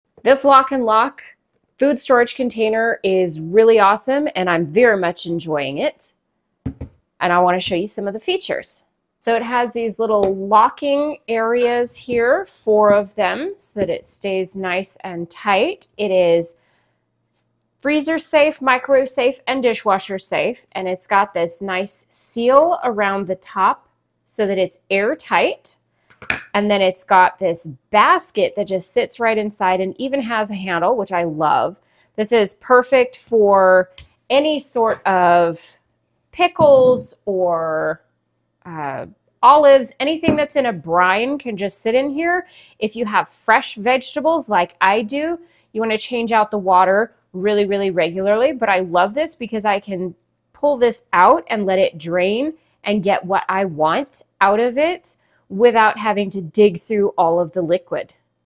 Airtight and Watertight Seal
In this video we review the Lock & Lock Pickle Container.